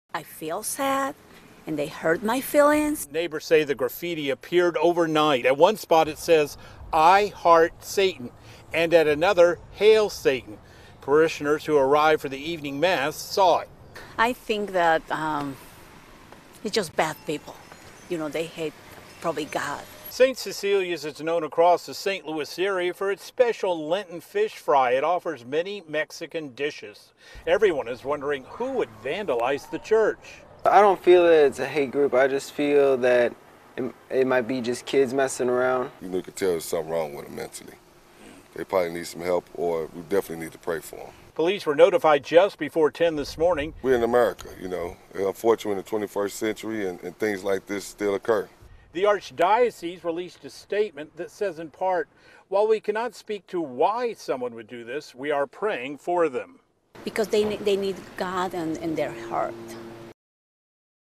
Vandals spray paint graffiti on South City church
First on FOX, a catholic church in south St. Louis was hit by vandals.  Hateful messages were spray painted on St. Cecilia`s overnight. The church, which has many Hispanic parishioners is known for its special fish fry`s during Lent. FOX 2/NEWS 11s Roche Madden was at the church with reaction from neighbors, the archdiocese and church members.